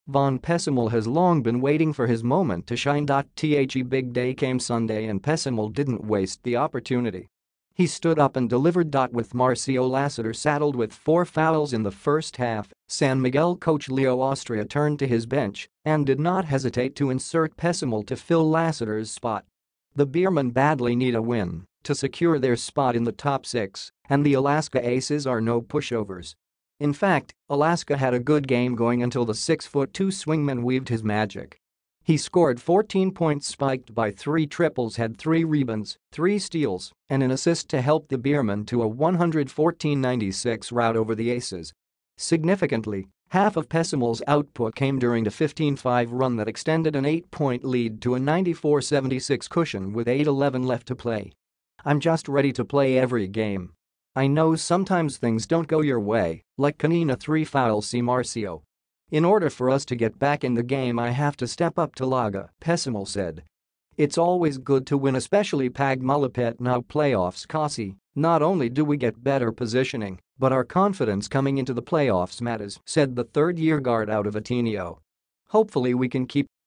Von Pessumal's moment to shine
Von Pessumal has long been waiting for his moment to shine.

The big day came Sunday and Pessumal didn't waste the opportunity. He stood up and delivered.

With Marcio Lassiter saddled with four fouls in the first half, San Miguel coach Leo Austria turned to his bench and did not hesitate to insert Pessumal to fill Lassiter's spot.

The Beerman badly need a win to secure their spot in the Top Six and the Alaska Aces are no pushovers.

In fact, Alaska had a good game going until the 6-foot-2 swingman weaved his magic. He scored 14 points -- spiked by three triples -- had three rebounds, three steals, and an assist to help the Beermen to a 114-96 rout over the Aces.

Significantly, half of Pessumal's output came during a 15-5 run that extended an eight-point lead to a 94-76 cushion with 8:11 left to play.

"I'm just ready to play every game. I know sometimes things don't go your way, like kanina three fouls si Marcio. In order for us to get back in the game I have to step up talaga," Pessumal said.

"It's always good to win especially 'pag malapit na playoffs kasi not only do we get better positioning but our confidence coming into the playoffs mataas," said the third-year guard out of Ateneo.

"hopefully we can keep going until the playoffs."